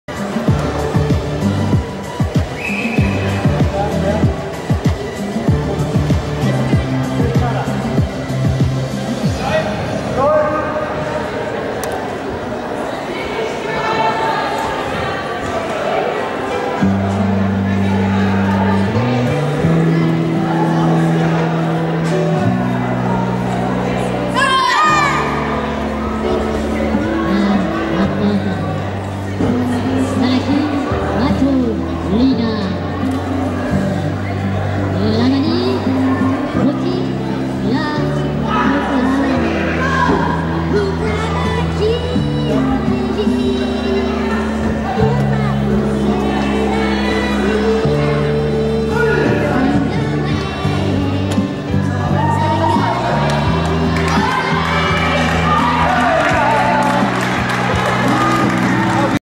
gkr wgtn r2 30 08 2014 ring2 jnr team kata 04a taigyoku shodan
GKR Karate
Wellington, New Zealand
Round 2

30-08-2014

Ring2: Junior Team Kata